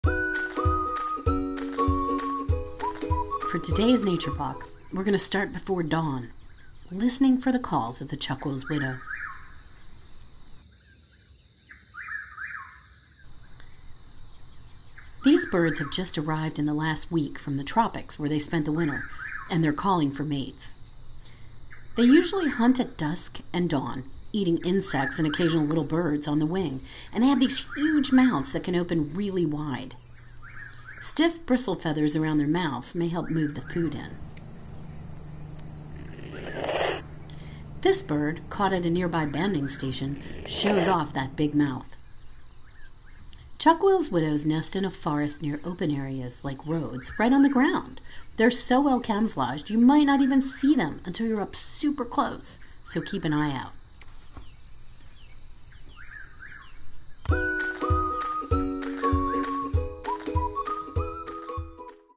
Chuck Wills Widow
Today’s nature walk starts before dawn, listening for the calls of the chuck wills widow. This is actually a species in pretty steep decline, so it’s always good to hear them.

These birds have just arrived in the last week from the tropics where they spent the winter, and they are calling for mates.

They usually hunt at dusk and dawn, eating insects (and occasional little birds) on the wing, and they have these huge mouths that can open really wide.  Stiff bristle feathers around their mouths may help move the food in. This bird was caught at the nearby banding station, and when it protested the indignity of banding you can see (and hear) that big mouth.

They’ll nest in a forest  near open areas (like roads) right on the ground.  They’re so well camouflaged you might not see them until you’re too close, so keep an eye out.